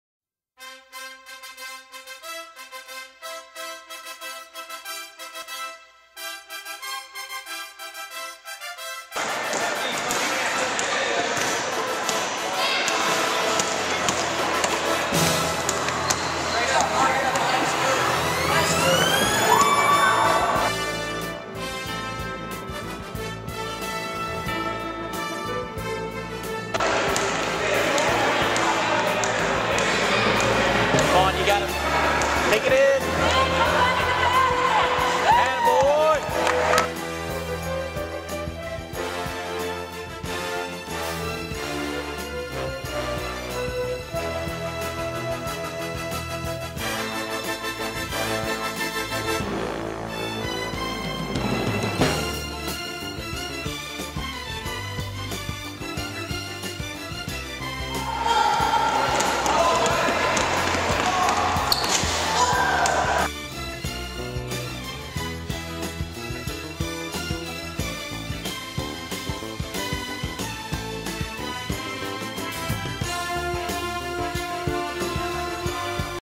Hoops! Caden Throneberry Age 6
Caden hooping it up ...St. Louis YMCA League ... Caden at age 6 giving it a go as he takes it the bucket!